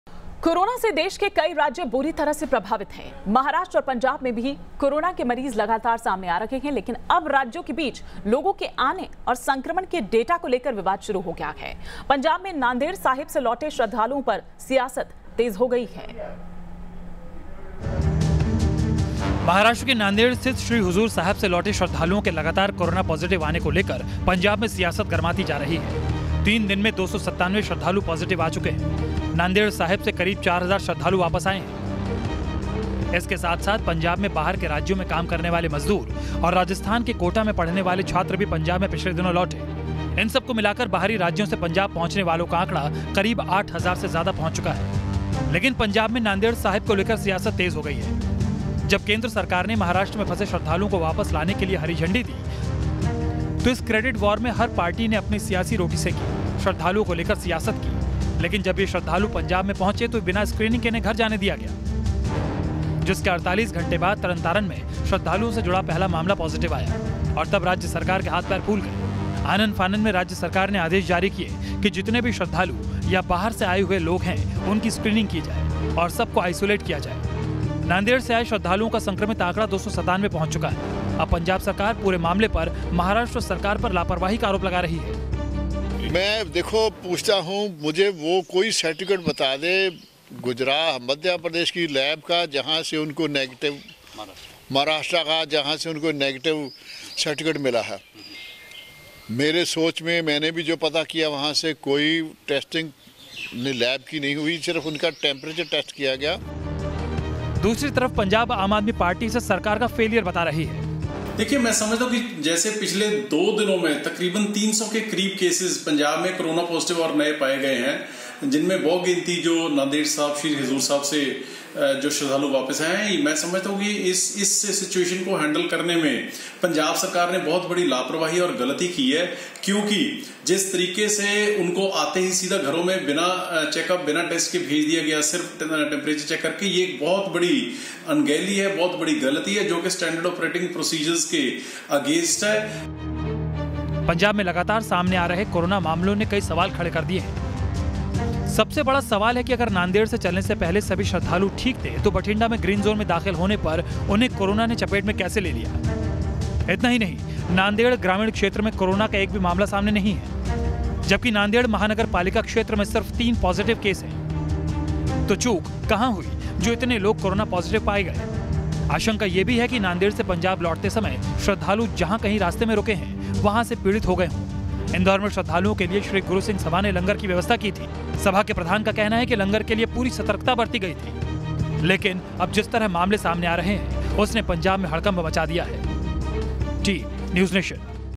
कोरोना पर महाराष्ट्र-पंजाब आमने-सामने, नांदेड़ श्रद्धालुओं पर सियासत हुई गर्म
पंजाब के मुख्यमंत्री कैप्टन अमरिंदर सिंह ने नांदेड से आए श्रद्धालुओं को लेकर महाराष्ट्र सरकार पर निशाना साधा. मुख्यमंत्री कैप्टन अमरिंदर सिंह ने कहा कि महाराष्ट्र सरकार ने नांदेड़ में फंसे श्रद्धालुओं के कोरोना टेस्ट को लेकर हमसे झूठ बोला. उन्होंने हमसे कहा था कि हम लोगों का कोरोना टेस्ट करवा चुके हैं, लेकिन श्रद्धालुओं का टेस्ट नहीं हुआ था. यदि हमें पता होता तो हम जरूर टेस्ट करवाते.

News State Apps डाउनलोड करें:
Download Android and iOS Apps for more updates: